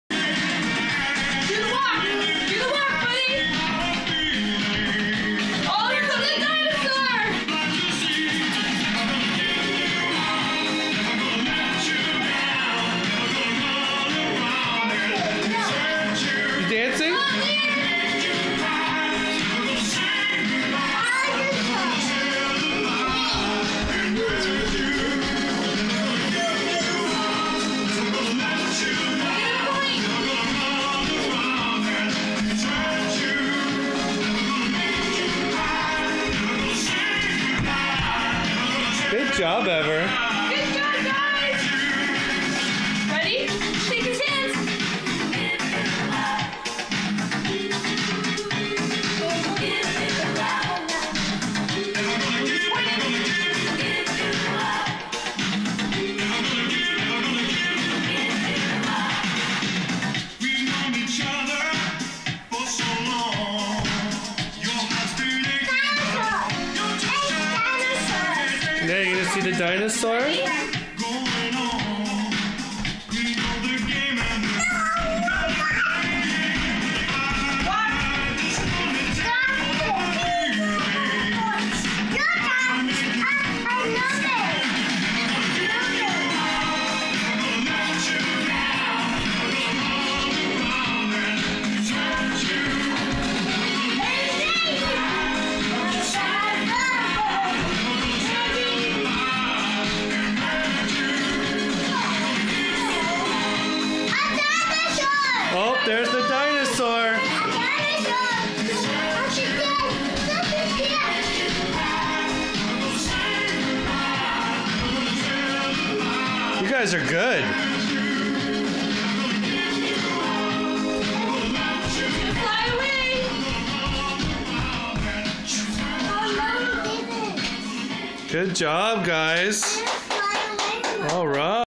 Breaking it on down to Rick Astley
The Maltese Falcons rippin' up the dancefloor to some Rick Astley.